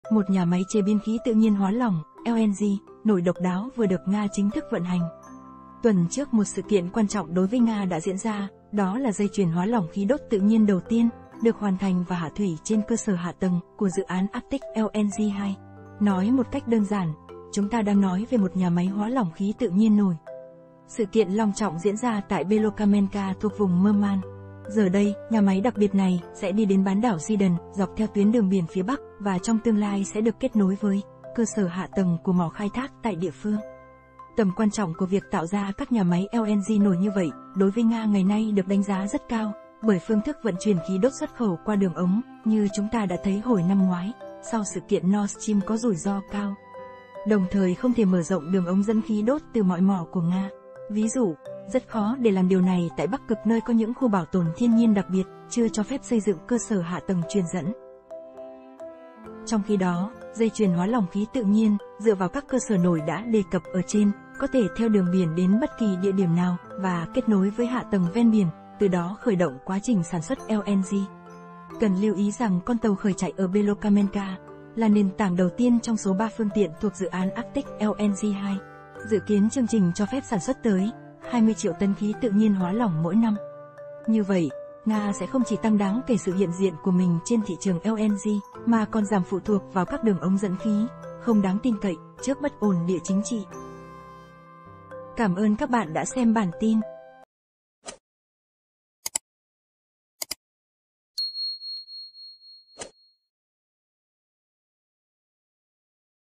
NGA CHÍNH THỨC VẬN HÀNH NHÀ MÁY LNG NỔI CÓ THỂ THEO ĐƯỜNG BIỂN ĐẾN BẤT KỲ ĐỊA ĐIỂM NÀO
Tuần trước một sự kiện quan trọng đối với Nga đã diễn ra, đó là dây chuyền hóa lỏng khí đốt tự nhiên đầu tiên, được hoàn thành và hạ thủy trên cơ sở hạ tầng, của dự án Arctic LNG 2. Nói một cách đơn giản, chúng ta đang nói về một nhà máy hóa lỏng khí tự nhiên nổi.